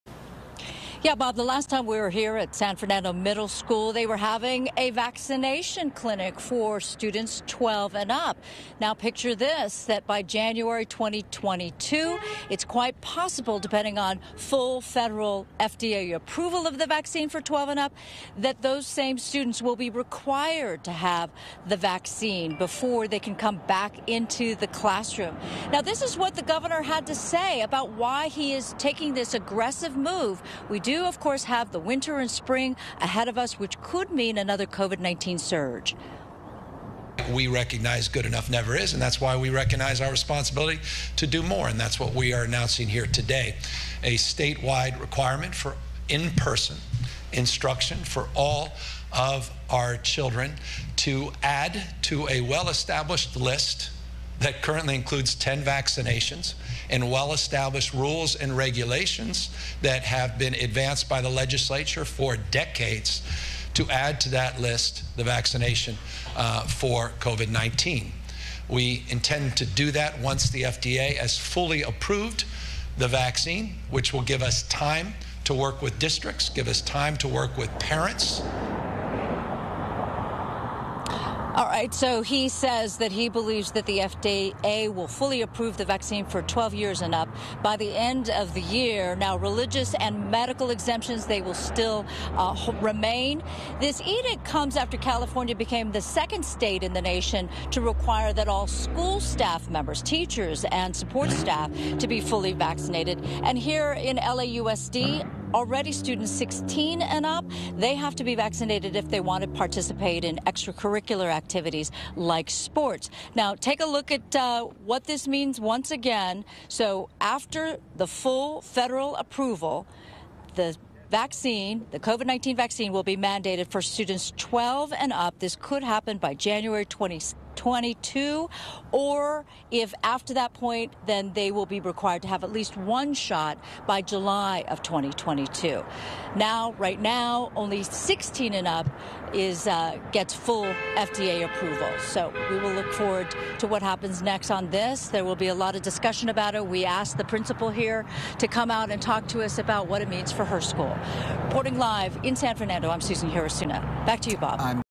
California becomes first state in nation to announce vaccine requirements for students
After mounting pressure from the community and school boards, California on Friday became the first in the nation to announce to require eligible students to be vaccinated for in-person learning.